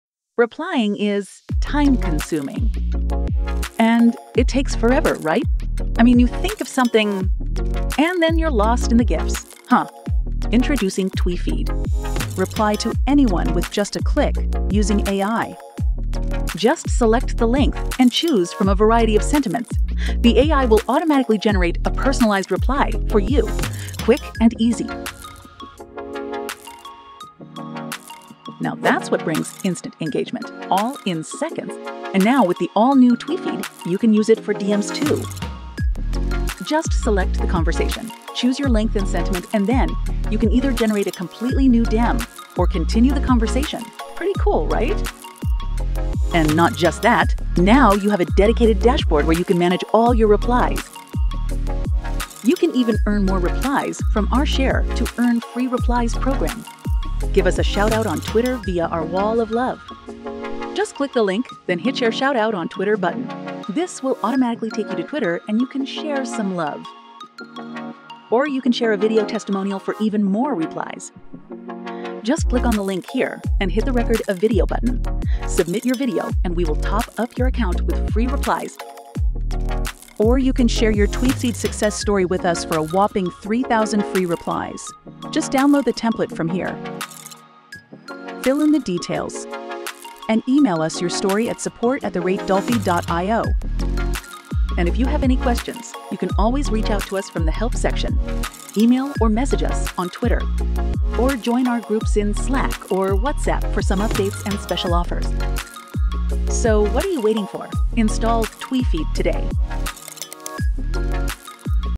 TweeFeed | Craft Perfect Tweet Replies & DM with GPT-4 AI
A game-changing chrome extension, TweeFeed, leverages GPT 4 Engine and data to revolutionize X (Twitter) engagement.

Craft personalized responses in seconds, manage brand accounts, boost sales, and initiate DM conversations with confidence. Say goodbye to missed opportunities with AI-powered response generation and unlock endless opportunities.

Key features:
1. Personalized responses during X (Twitter) interactions
2. Different replies for each mood
3. Time-saving one-click reply generation
4. Analyzing user profiles to create powerful first DM
5. Personalized DM suggestions to continue conversations